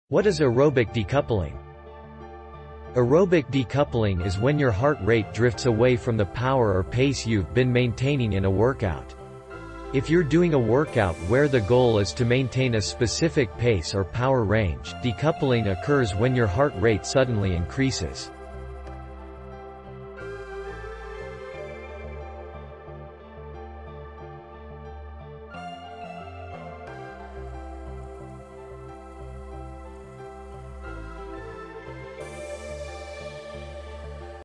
What is aerobic decoupling?
Aerobic decoupling is when your heart rate drifts away from the power or pace you’ve been maintaining in a workout. ... If you’re doing a workout where the goal is to maintain a specific pace or power range, decoupling occurs when your heart rate suddenly increases.